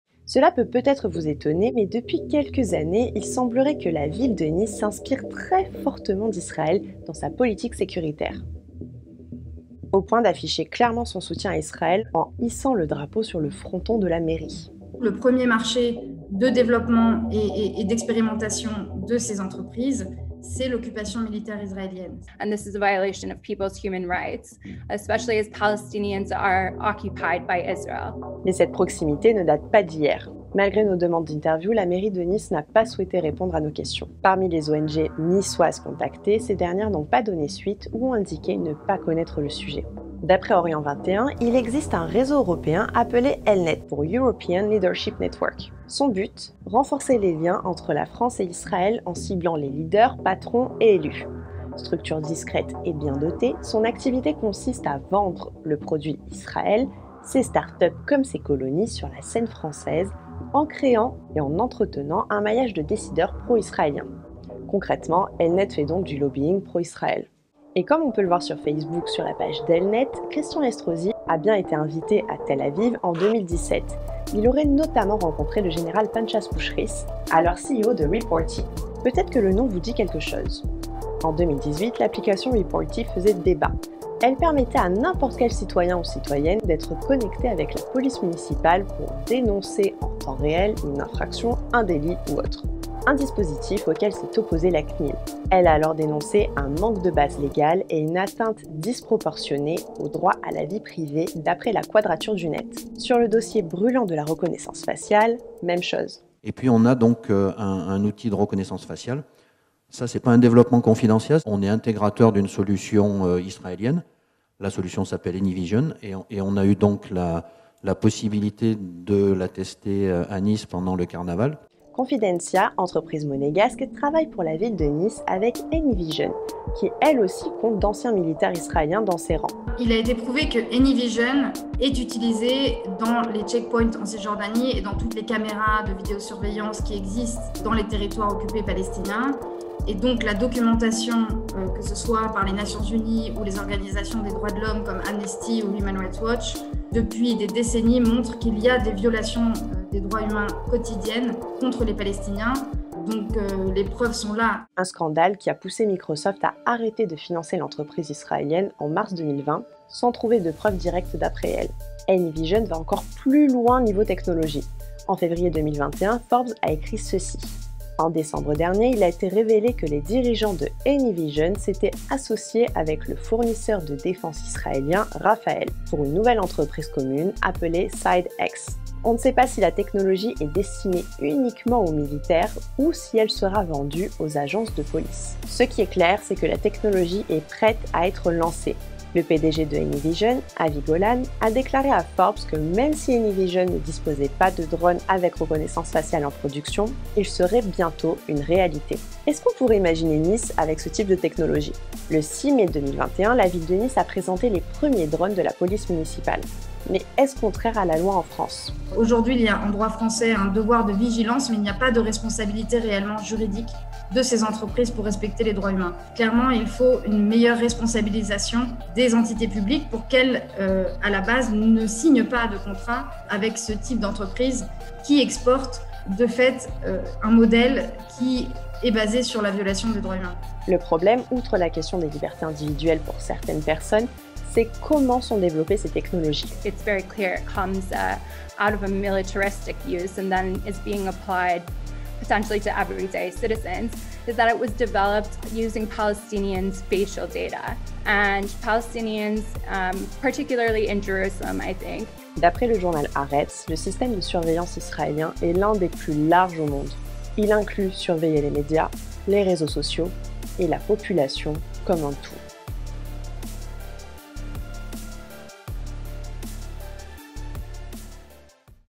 Nice, Israël et sa politique sécuritaire
Quel est le lien entre la ville de Nice, start-up israéliennes et droits de la population palestinienne ? De prime abord, vous ne voyez peut-être pas le lien. Mais regardez du côté de la high-tech sécuritaire. Pourquoi ? On vous explique.